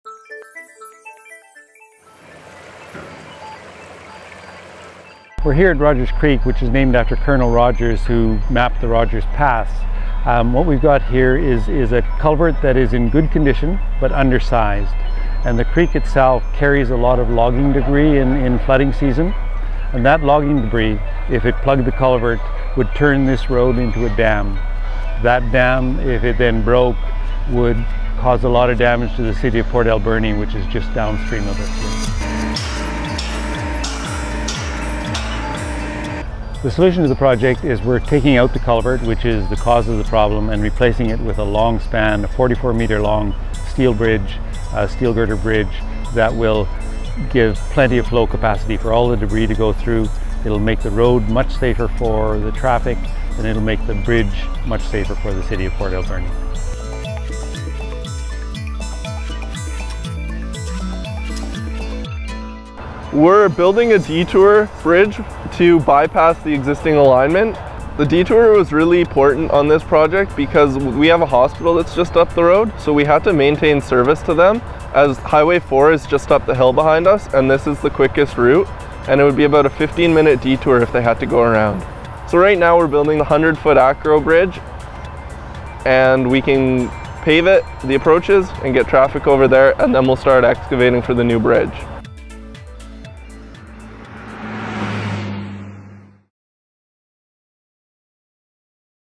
Bridging Rogers Creek to Protect Port Alberni
There's a lot of digging and bridging going on at Rogers Creek in Port Alberni.

The ministry is replacing the culvert, which runs under the Port Alberni Highway, with a 44-metre-long single span steel girder bridge. The bridge will provide a wider opening for Rogers Creek to pass through.

Why wasn't the culvert good enough? It was relatively small in comparison to some of the logging debris that builds up. In fact, during a 2006 storm, emergency crews needed to respond quickly to clear heavy logging debris before potentially triggering a series of damaging events.

"That logging debris, if it plugged the culvert, would turn this road into a dam," says project manager Allan Galambos. "That dam, if it then broke, would cause a lot of damage to the city of Port Alberni, which is just downstream."

With the local hospital nearby, it's important to keep traffic flowing while crews remove the culvert and install the bridge. That's why contractor Neelco Builders Ltd. recently installed a temporary detour bridge to allow vehicles to pass next to the construction site.

TranBC visited the site last month while the temporary bridge was being installed, piece by piece, and collected some video footage to help explain what's going on to travellers. Take a look! And if you do drive through during construction, please watch for workers and traffic controllers. Construction is expected to be completed by fall 2014.